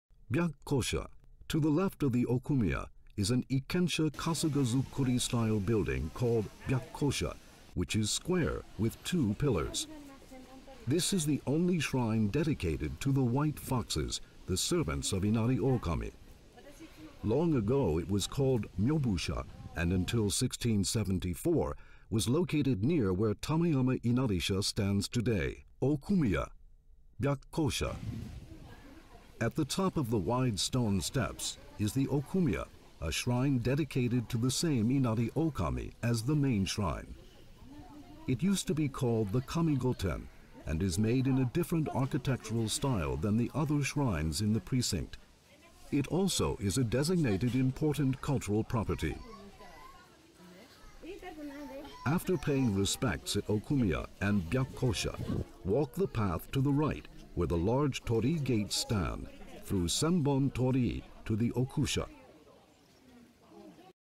Byakkosha, Okumiya Fushimi Inari Audio guide(Kyoto)
To the left of the Okumiya is an Ikkensha Kasugazukuri-style building called Byakkosha which is square with two pillars. This is the only shrine dedicated to the “white foxes,” the servants of Inari Okami. Long ago, it was called Myobusha and until 1674 was located near where Tamayama Inarisha stands today. This building is also a designated important cultural property.
After paying respects at Okumiya and Byakkosha, walk the path to the right where the large torii gates stand, through Senbon Torii to the Okusha.

At the top of the wide stone steps is the Okumiya, a shrine dedicated to the same Inari Okami as the main shrine. It used to be called the Kamigoten and is made in a different architectural style than the other shrines in the precinct. It also is a designated important cultural property.